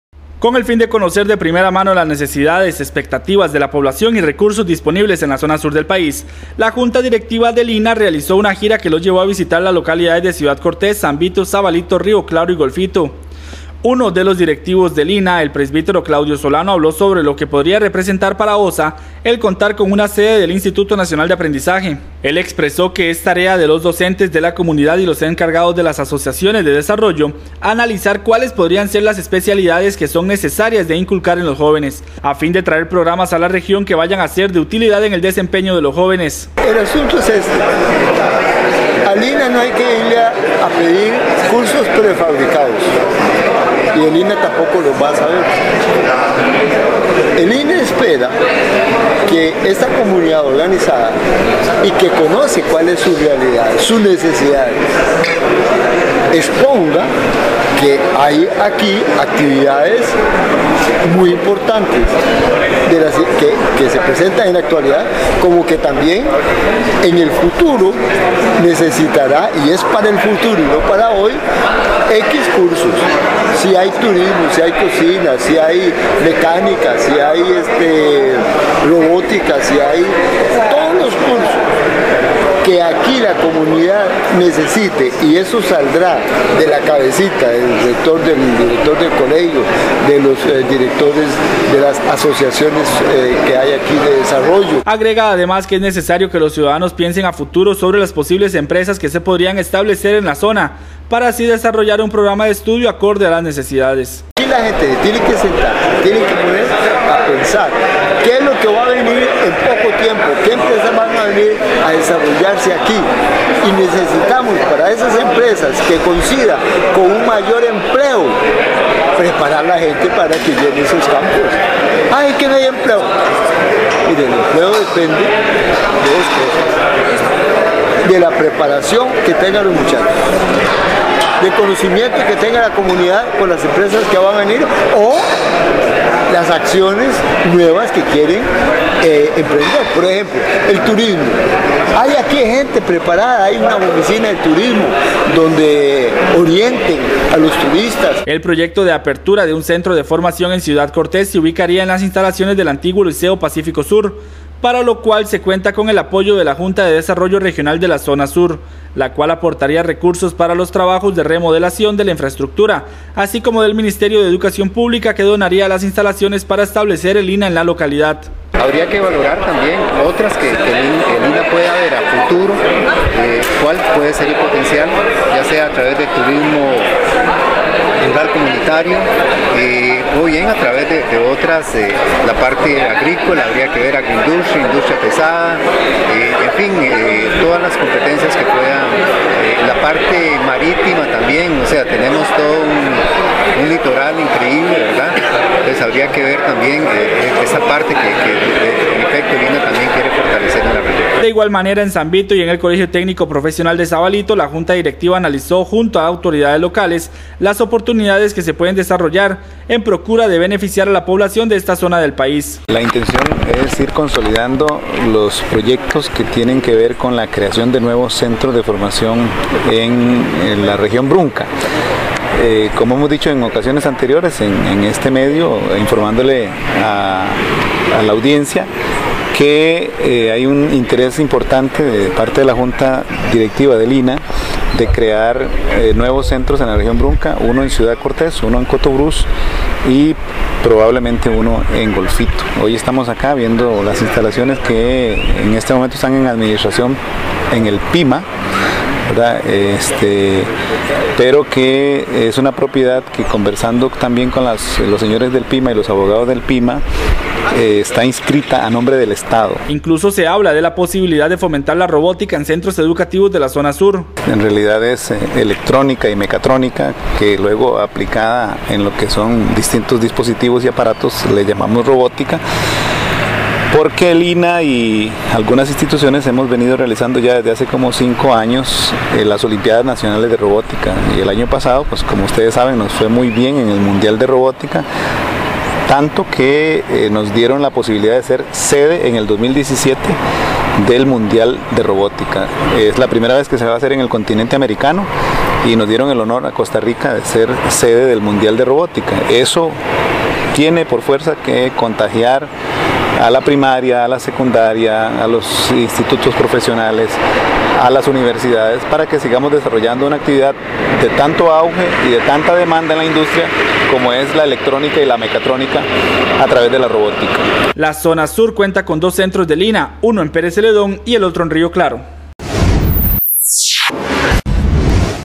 La Zona Sur podría tener más centros del INA
Con el fin de conocer de primera mano las necesidades, expectativas de la población y recursos disponibles en la zona sur del país, la Junta Directiva del INA realizó una gira que los llevó a visitar las localidades de Ciudad Cortés, San Vito, Sabalito, Río Claro y Golfito.